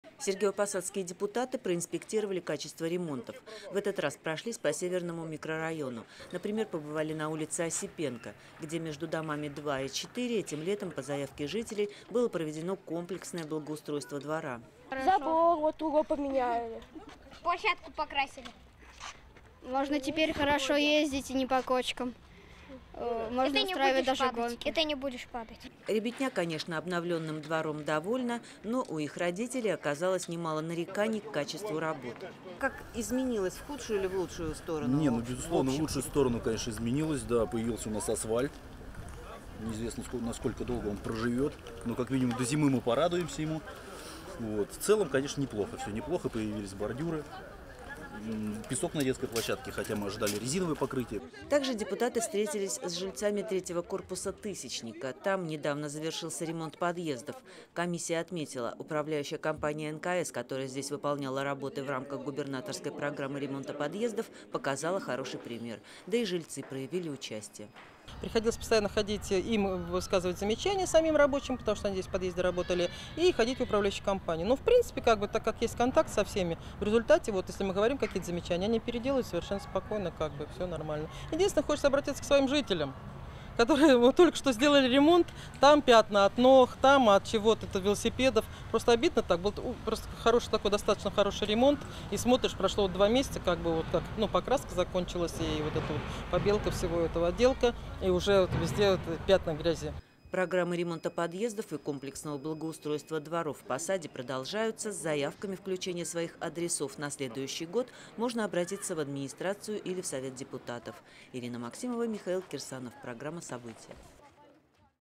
В «тысячнике» отремонтировали подъезды | Новости | ТВР24 | Сергиев Посад
13 августа 2018 | Сергиевопосадские депутаты проинспектировали качество ремонтов. В этот раз прошлись по Северному микрорайону. Например, побывали на улице Осипенко, где между домами №№2 и 4 этим летом по заявке жителей было проведено комплексное благоустройство двора.